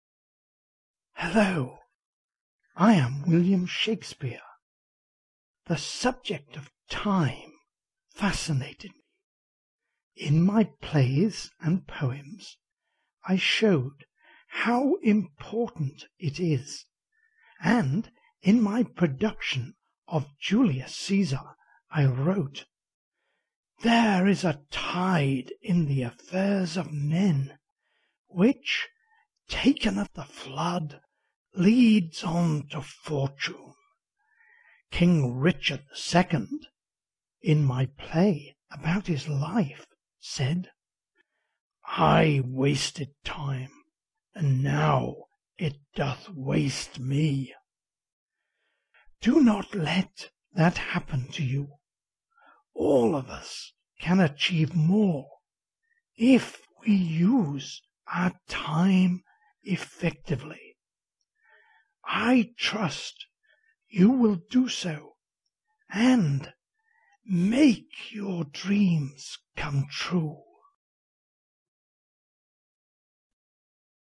William Shakespeare - Content Marketing sample
This is to show that there are ways and means to allow amazing people to represent just about any brand in a positive and unique way.

Big brands want to associate their products with something or someone positive and The Amazing People Club has the content and support resources available to provide unique and endless possibilities for amazing campaigns.   The content can be tailored to enhance and support any brand, large or small and align it with amazing characters.